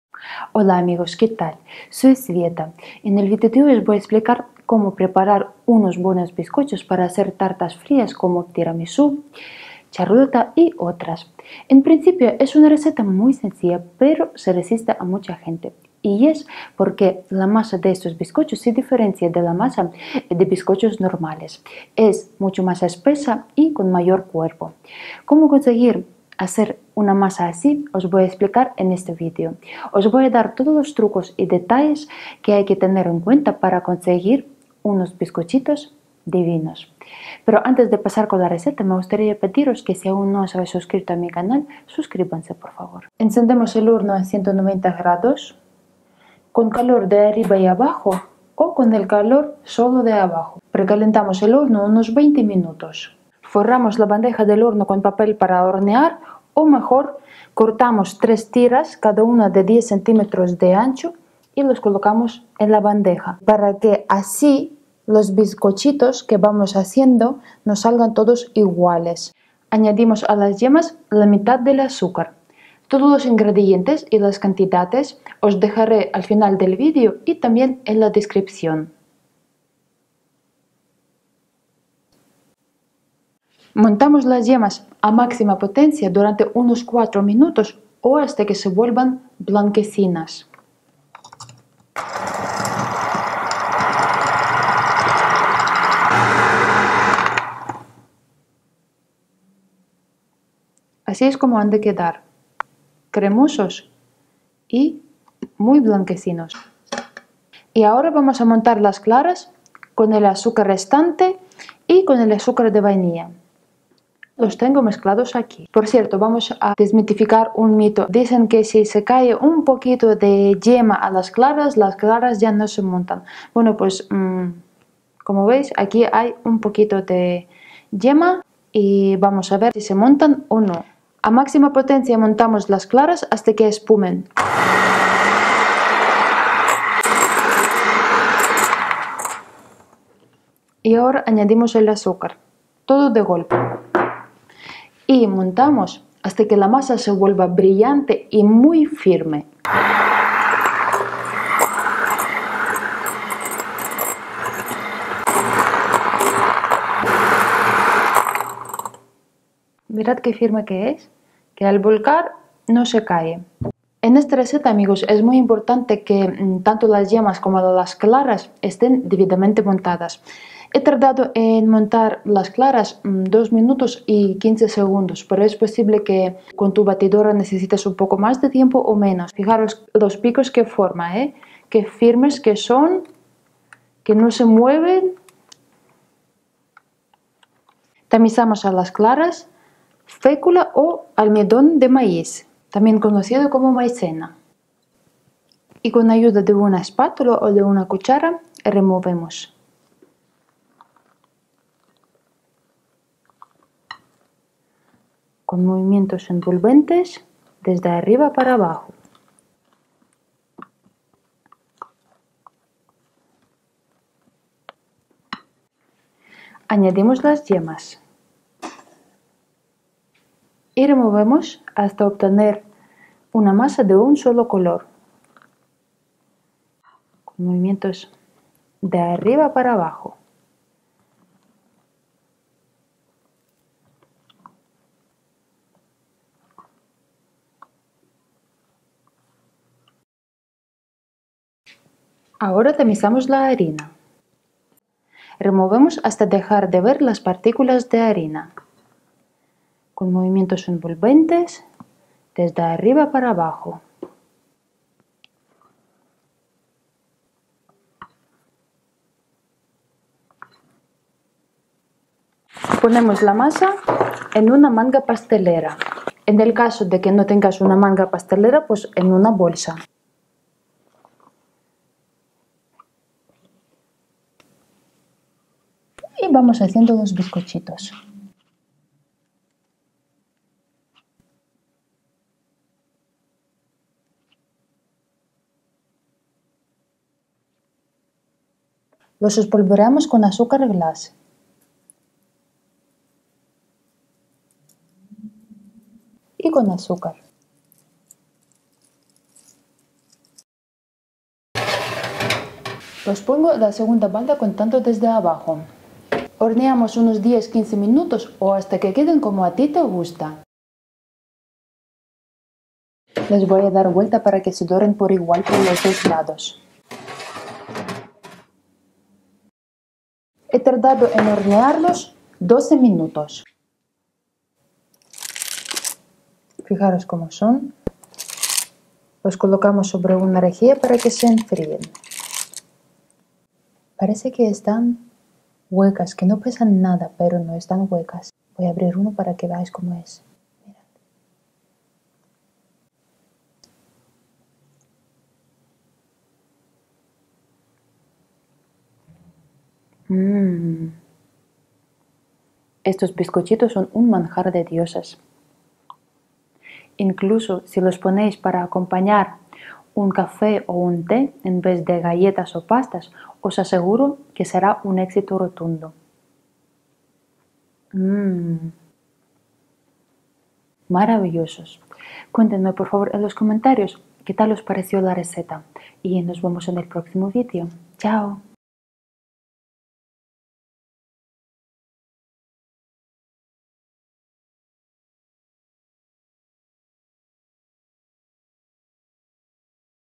Bizcochos para hacer TIRAMISÚ, carlota y otros postres fríos - Savoiardi
Receta de bizcochos savoiardi esponjosos. También conocidos como vainillas, melindres, lady fingers o bizcochos de soletilla. Estos bizcochos harán subir a tus postres fríos a un nivel superior ya que su sabor, textura y ligereza son excepcionales. También son ideales para acompañar café o té. Para las meriendas y desayunos. Para agasajar a los invitados y niños. Una receta que te hará brillar ante ojos de los demás.

Ingredientes:

3 yemas
2 claras
35 g de harina de trigo de todo uso
30 g de almidón o fécula de maiz
50 g de azúcar + 1 sobre de 8 g de azúcar de vainilla
ó 60 g de azúcar + esencia de vainilla
1 pizca de sal

Sube tu foto al Instagram y etiquetame para que la vea y te pondré un corazón: recetas_de_esbieta